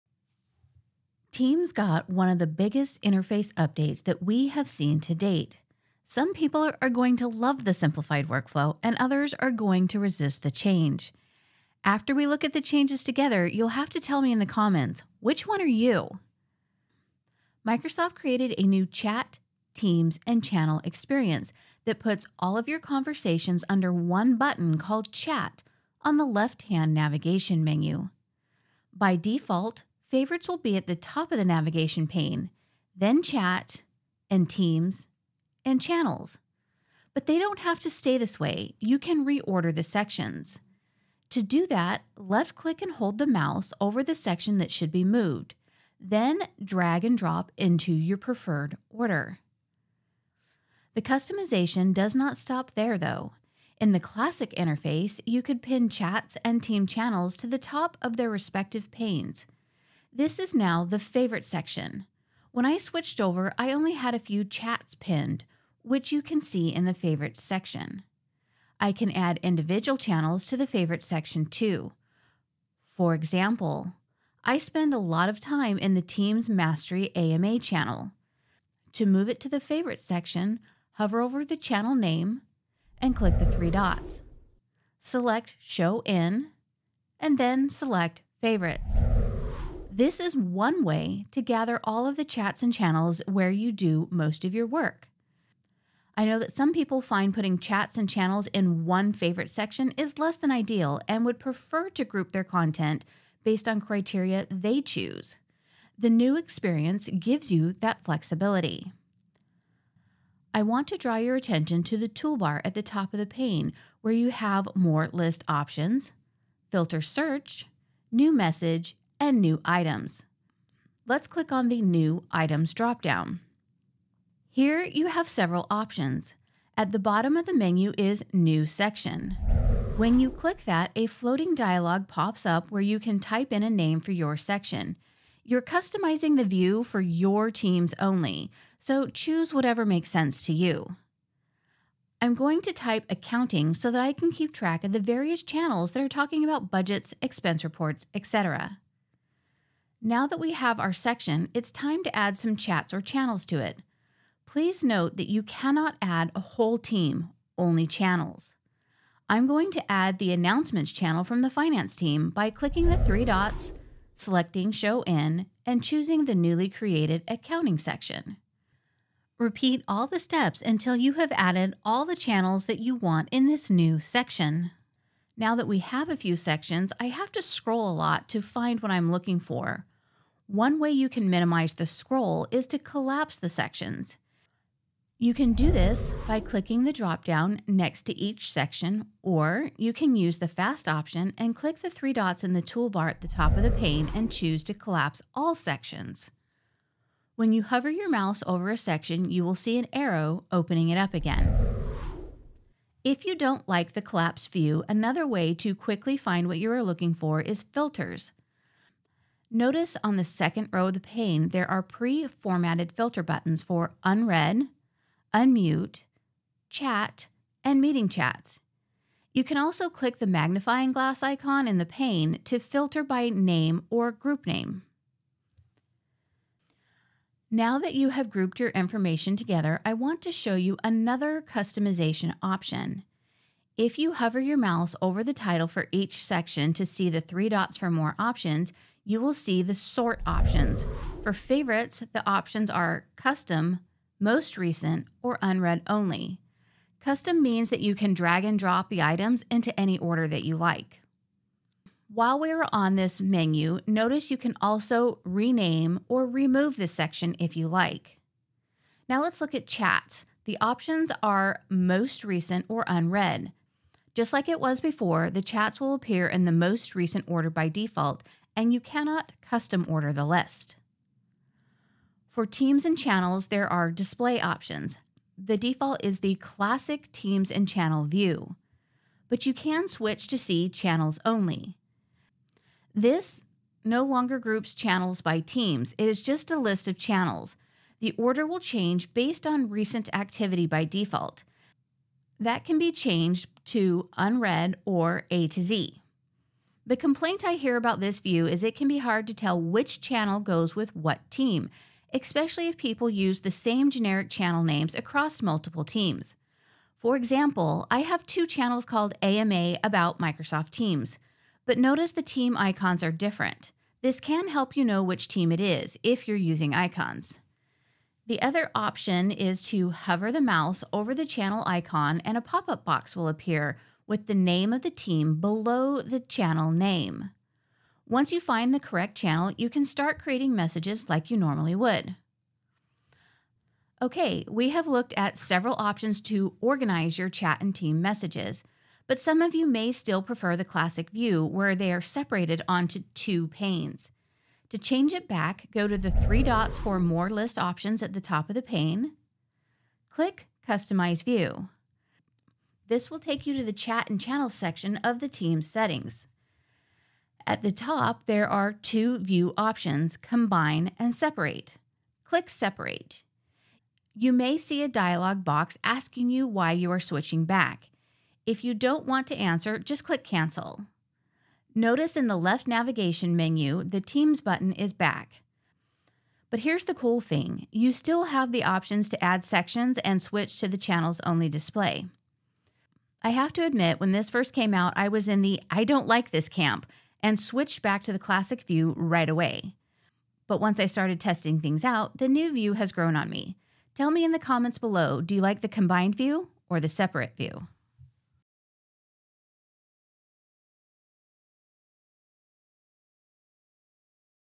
Master the New Teams Chat and Channel Experience
This tutorial will demonstrate how to use the new Teams chant and Channel Experience. The big thing to know up front is that the look and feel of interface is changing. Instead of having two buttons on the navigation menu for Chat and Teams it will be combined into one button called Chat.

Some people will love having all their messages in one place, while others will hate it. But don’t worry, I’ve got your covered. We will look at how to use the new interface and organize your messages to fit your workflow. If it is not for you, I will demonstrate how to find the setting to go back to having Teams and Chats separate.